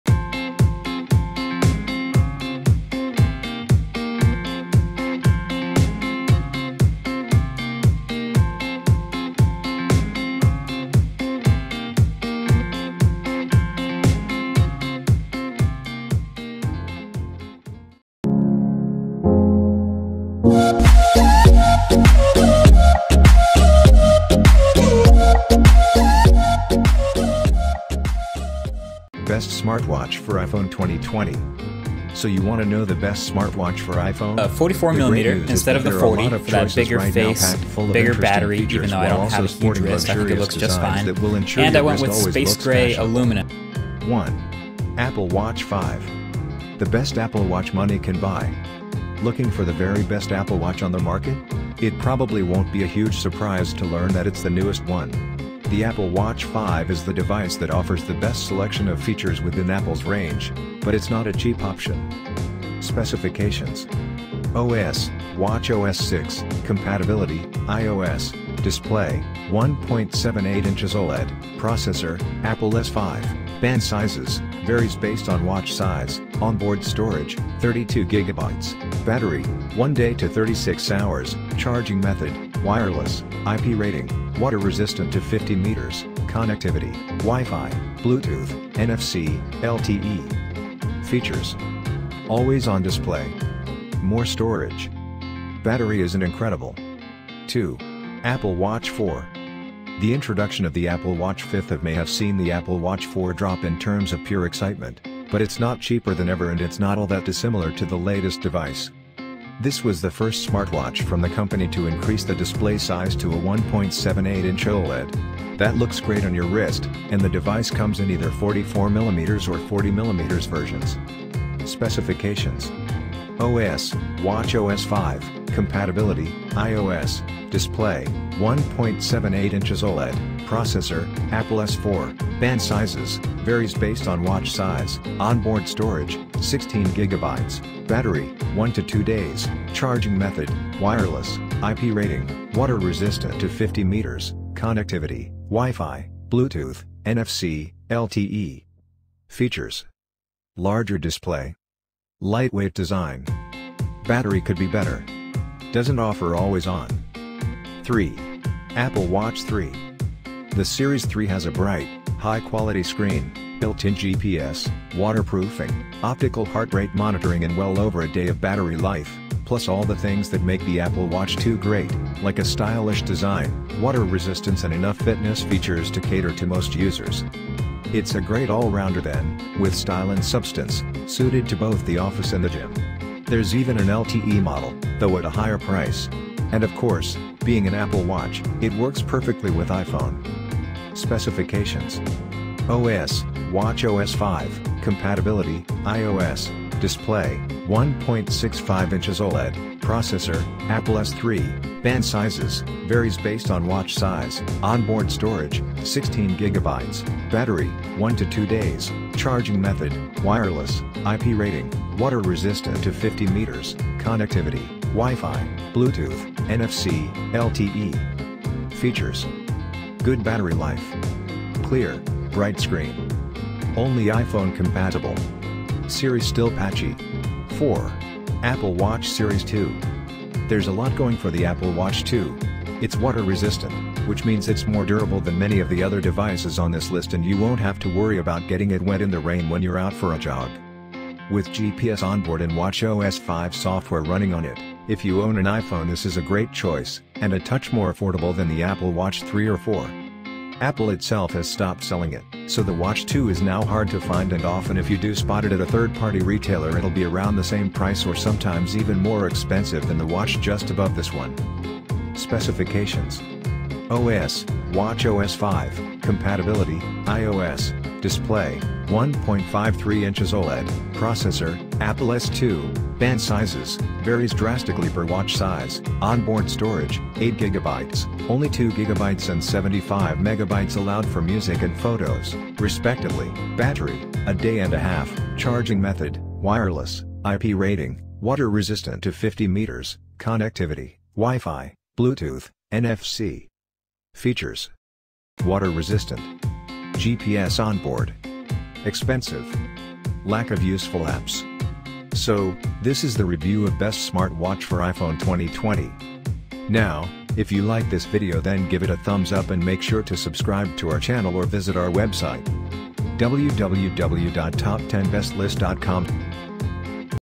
5 Best Smart Watches for iPhone in 2020 (UPDATED LIST)
So you want to know the Best Smart Watches for iPhone? The great news is that there are a lot of choices right now packed full of interesting features while also sporting luxurious designs that will ensure your wrist always looks fashionable.

The bad news is you're going to have a very hard decision to make choosing the best smart watch to use with your iPhone.

So, that's why here we have shared the list of the Best Smart Watches for iPhone in 2020.